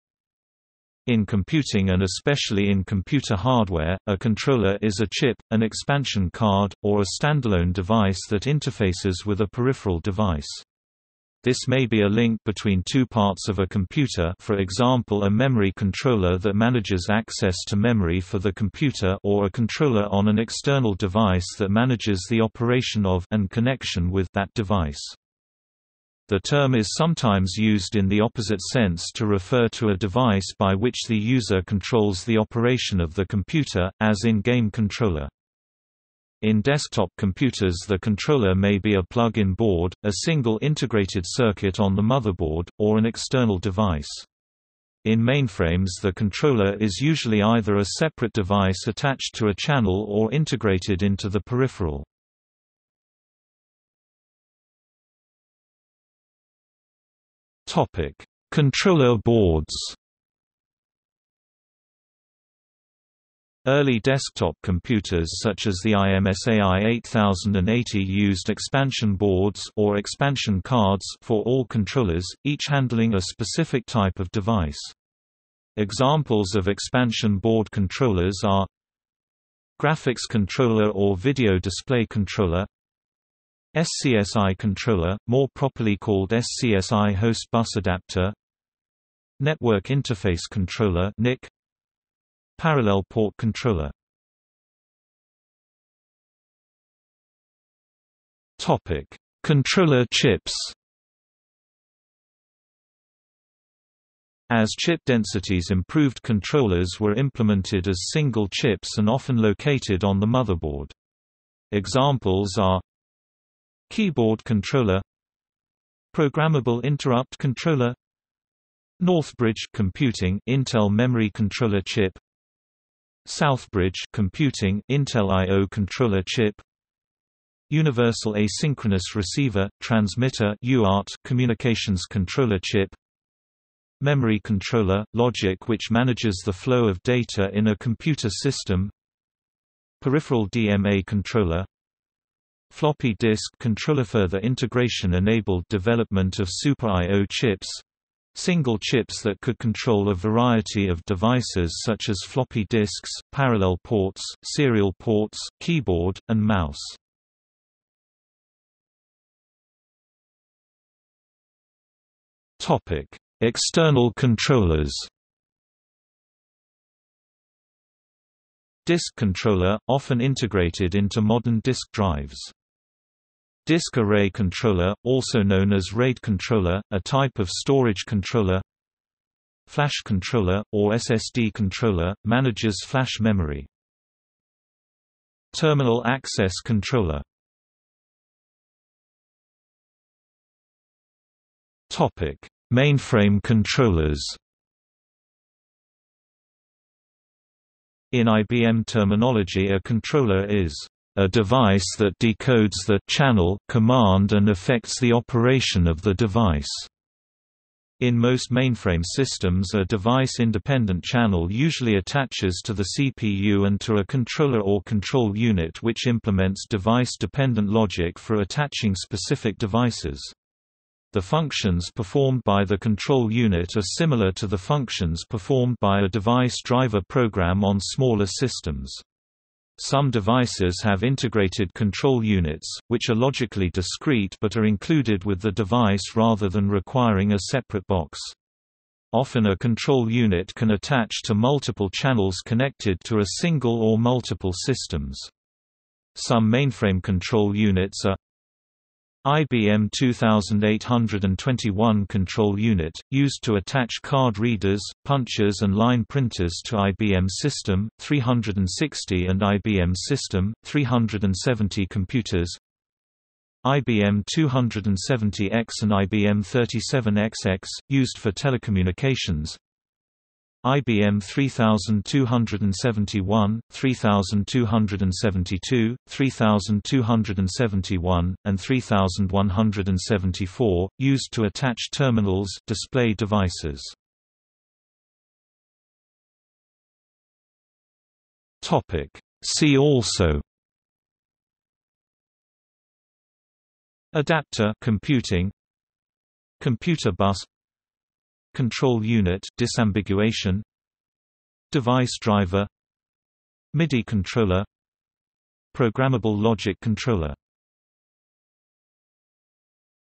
Controller (computing) | Wikipedia audio article
00:00:54 1 Controller boards
00:01:34 2 Controller chips
00:02:38 3 External controllers
00:03:08 4 Mainframe controllers
00:04:45 5 See also

- Socrates

SUMMARY
In computing and especially in computer hardware, a controller is a chip, an expansion card, or a stand-alone device that interfaces with a peripheral device. This may be a link between two parts of a computer (for example a memory controller that manages access to memory for the computer) or a controller on an external device that manages the operation of (and connection with) that device.
The term is sometimes used in the opposite sense to refer to a device by which the user controls the operation of the computer, as in game controller.
In desktop computers the controller may be a plug in board, a single integrated circuit on the motherboard, or an external device.  In mainframes the controller is usually either a separate device attached to a channel or integrated into the peripheral.